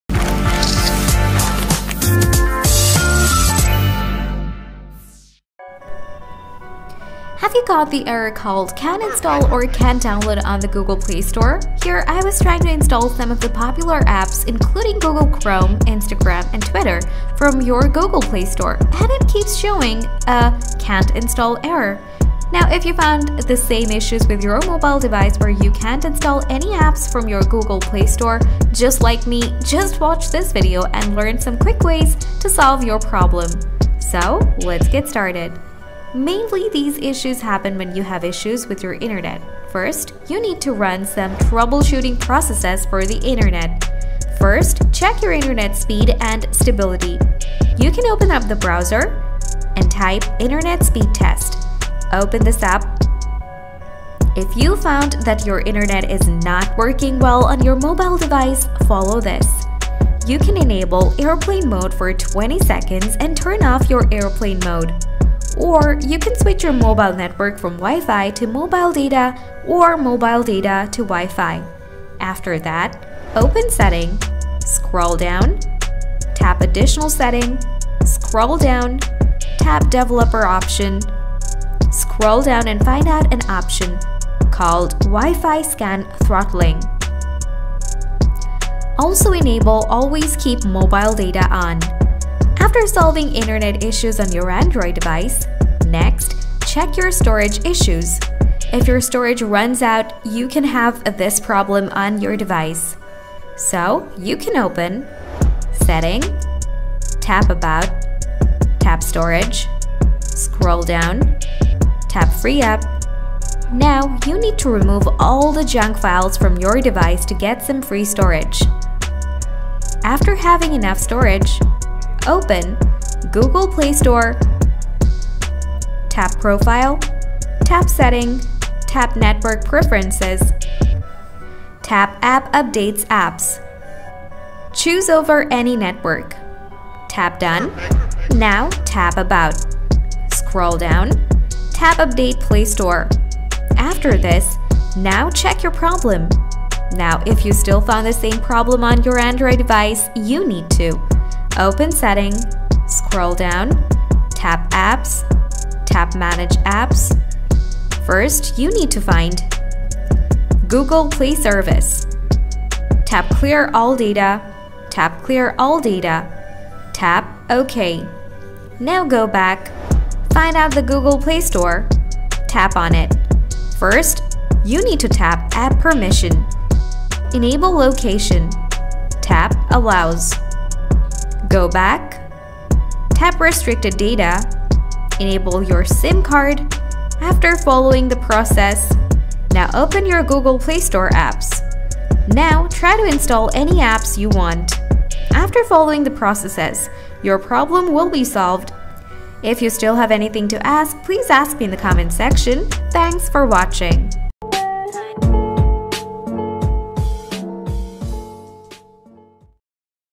How To Fix Can't Install App Issue On Play Store | Solve Can’t Install App Error
Are you facing apps not installing issue on play store? To can’t install app error on play store, get in touch with the video.
Here I was trying to install some of the popular apps, including google chrome, Instagram, and Twitter, from your google play store, and it Keeps showing can’t install app error or can’t install app issue on play store. To  Solve Can't Install or app not installing App Problem On Play store Android then, let's learn a quick fix!
having the problem, I would like to give you a practical solution that will fix the can’t install app error on play store Android!

00:01- Video intro
00:38- Solution 1: Check your internet speed
01:00- Solution 2: Fix the internet issue if you have the problem
01:14- Solution 3: Enable some settings from the developer option
01:38- Solution 4: Check your storage issue and fix it
02:05- Solution 5: Fix some settings from the play store
02:32- Solution 6: Follow the troubleshooting process
03:20- Solution ending